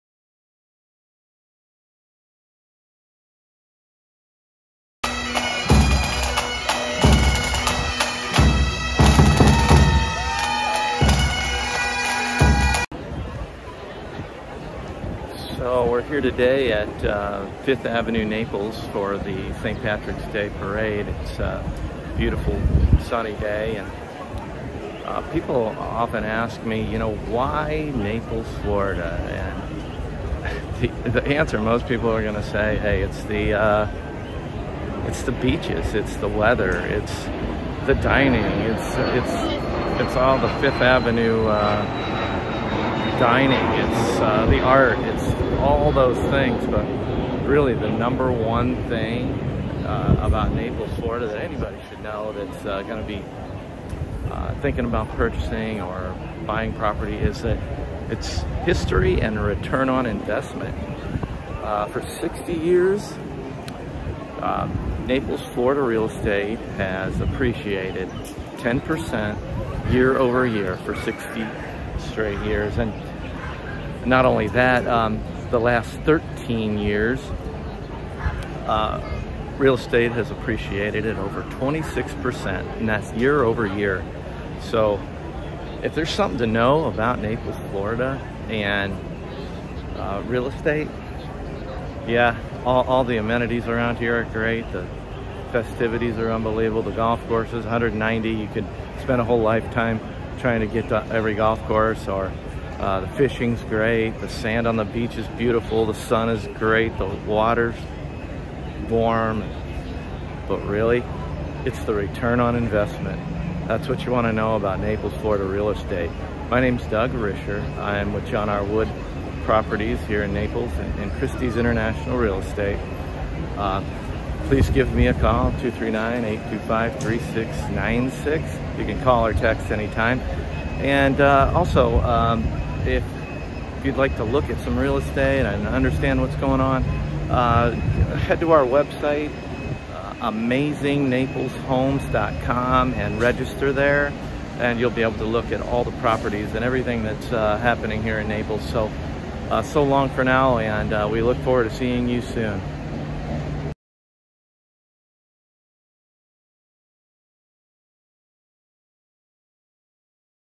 Why Naples Florida Real Estate? Return On Investment
Naples Florida Real Estate expected appreciation rates and return on investment.